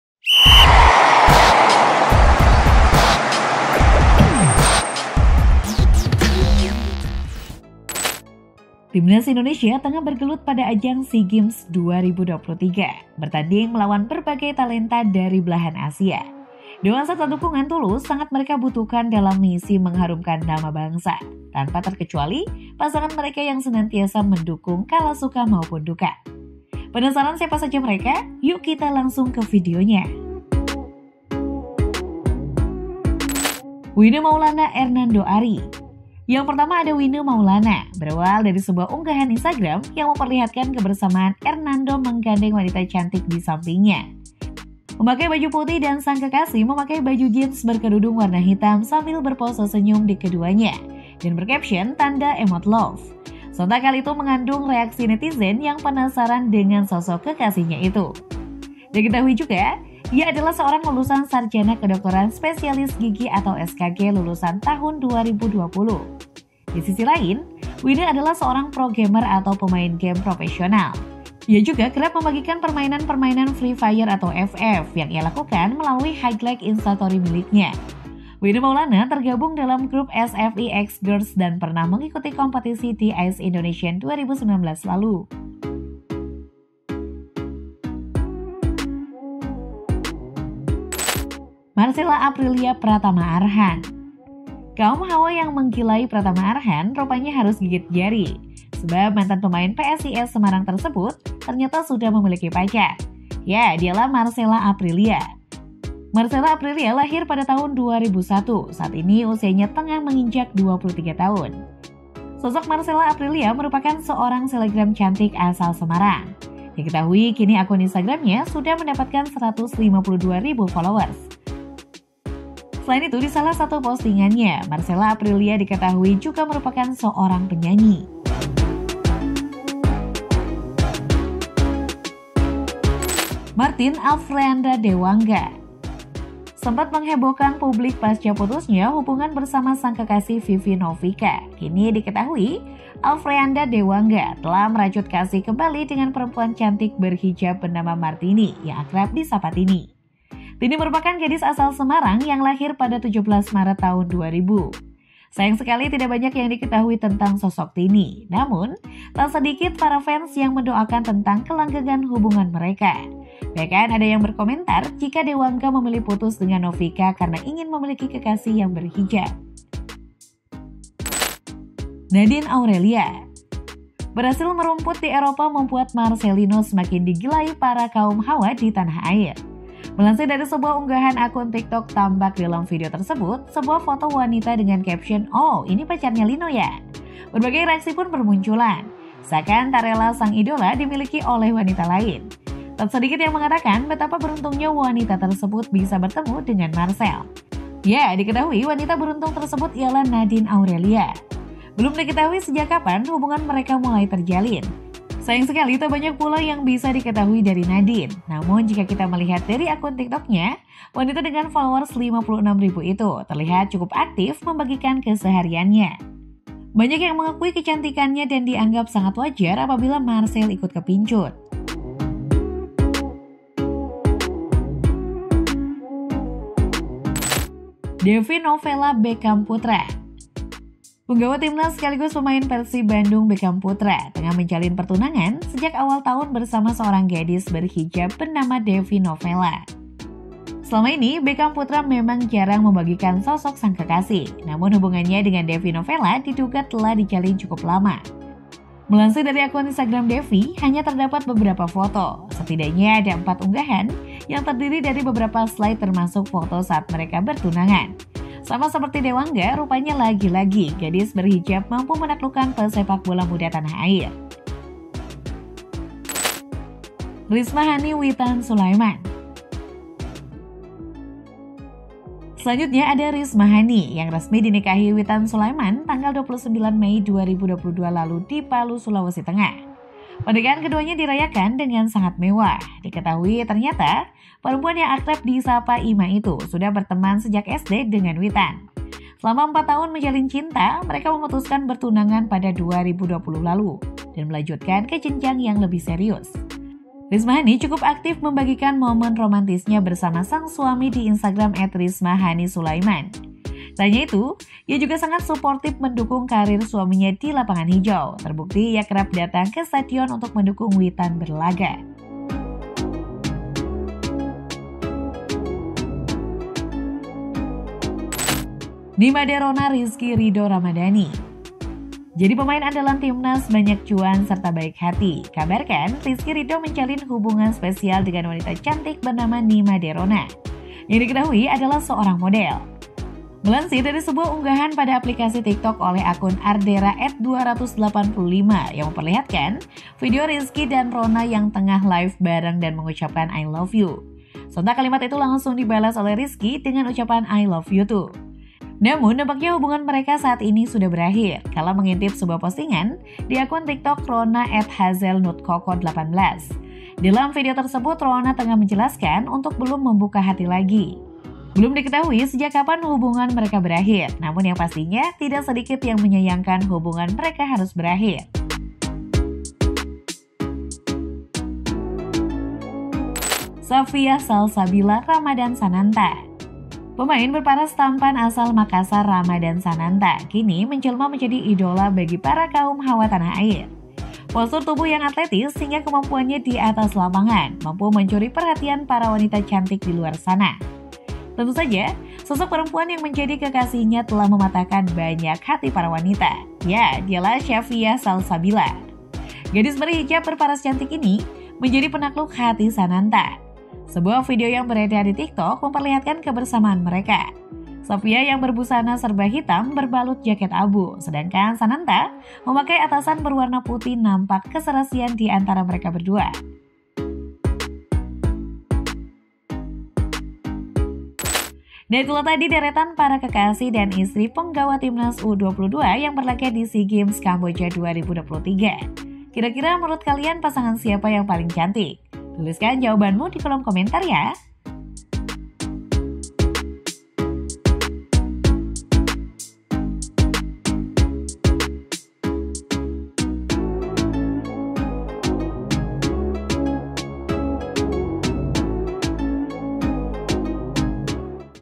SUKSES MEMBAWA TIMNAS JUARA SEA GAMES 2023! Inilah Pasangan Pemain Timnas Indonesia
Timnas Indonesia tengah bergelut pada ajang Sea Games 2023. Bertanding melawan berbagai talenta dari belahan asia, doa serta dukungan tulus sangat mereka butuhkan dalam misi mengharumkan nama bangsa, tanpa terkecuali pasangan mereka yang senantiasa mendukung kala suka maupun duka. penasaran siapa saja mereka? yuk kita langsung ke videonya